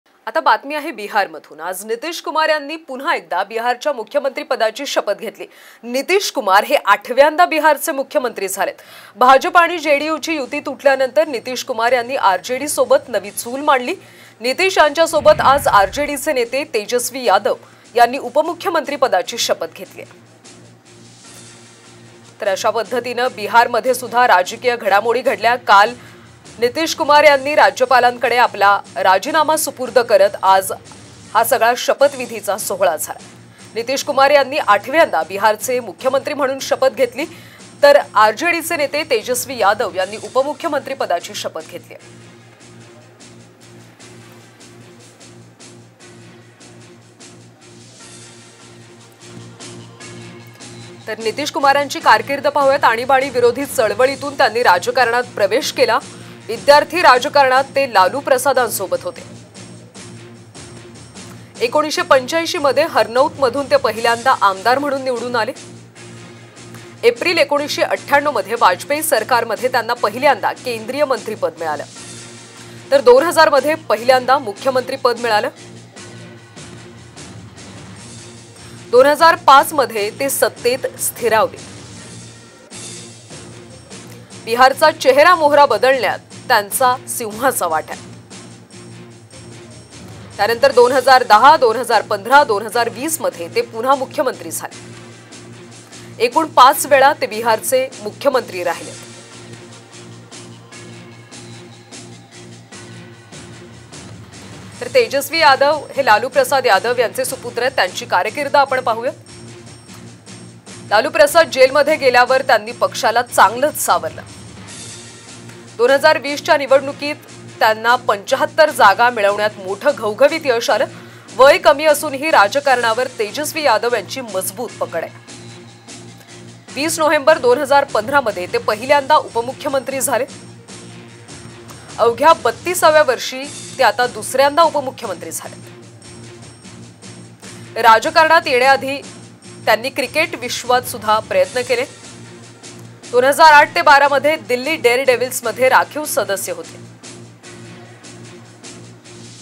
Bihar Politics Updates | नितीश कुमार मुख्यमंत्री तर तेजस्वी यादव उपमुख्यमंत्रीपदी विराजमान !
Watch The Latest News on Bihar Politics Updates | नितीश कुमार मुख्यमंत्री तर तेजस्वी यादव उपमुख्यमंत्रीपदी विराजमान !;पाहा व्हिडीओ